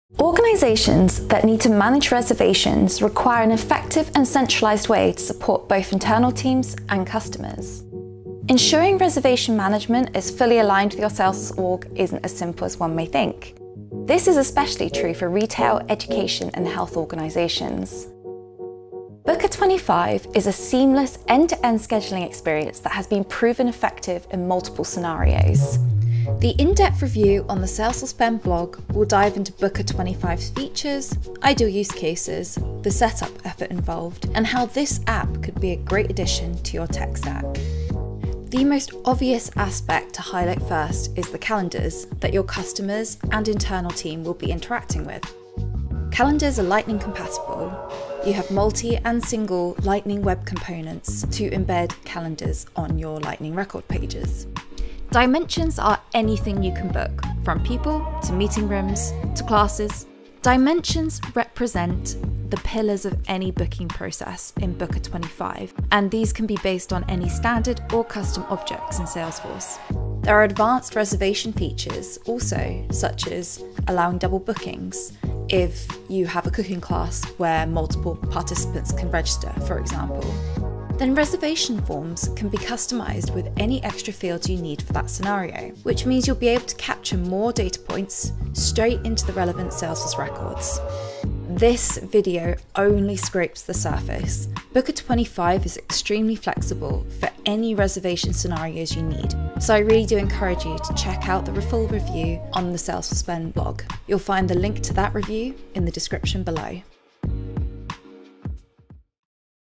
Native Booking App for Salesforce [In-Depth Review]
Booker25 is a seamless end-to-end scheduling experience. Built on the Salesforce platform, this application can be customized to meet any organization’s specific requirements. Already implemented by various companies, Booker25 is effective in multiple scenarios. It is also a more cost-effective option, guaranteed to improve your existing processes.

This in-depth review will dive into Booker25’s features, ideal use cases, and setup effort, as well as the different ways this app could be a great addition to your Salesforce org.